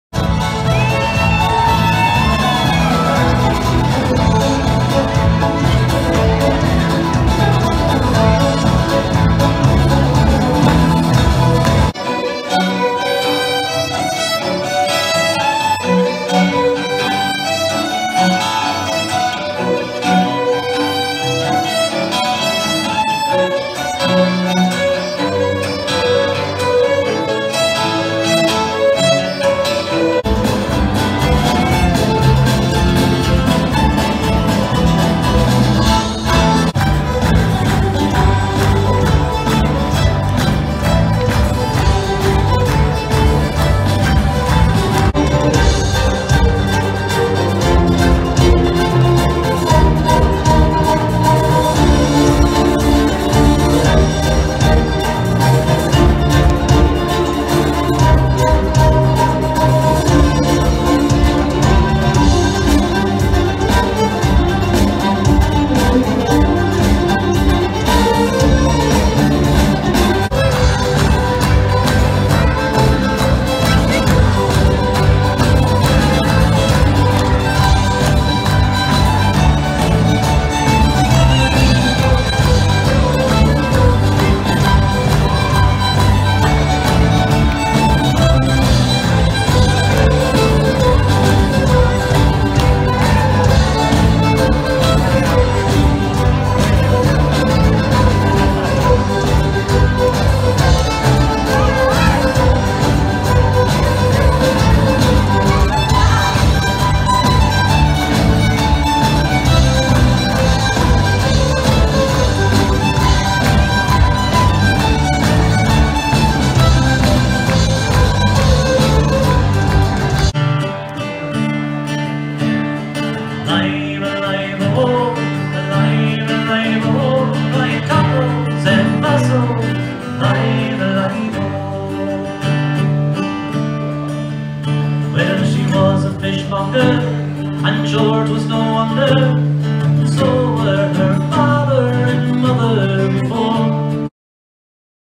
Lancaster Celtic Festival – in February?!?
The Rose & Shamrock festival takes place in the city of Lancaster over a weekend in mid-February.  It is centered at the downtown Marriott Convention Center, but events are held at a number of venues, including Annie Bailey’s, Tellus360, Meduseld Meadery, the Ware Center, Quips Pub and Leavengoods.

In addition to live Celtic bands, the festival includes an Irish dance competition that attracts around 900 dancers (from 94 different Irish dance schools in 15 states plus Canada in 2019), Irish music competition (on instruments such as the fiddle, harp, and tin whistle), Irish art competition, Irish baking competition, and a bagpiping competition.

All the clips in this video were filmed Friday, Feb.14, 2020.  The band is the Celtic Martins (also featured in one of my earlier videos Near the end, singer Dave Pedrick plays at Tellus360, a great Irish pub with bars and lounges on the basement level, first floor, second floor, and rooftop. The end photo shows Penn Square, the center of downtown Lancaster; I took it from an upstairs window of the Marriott that same evening.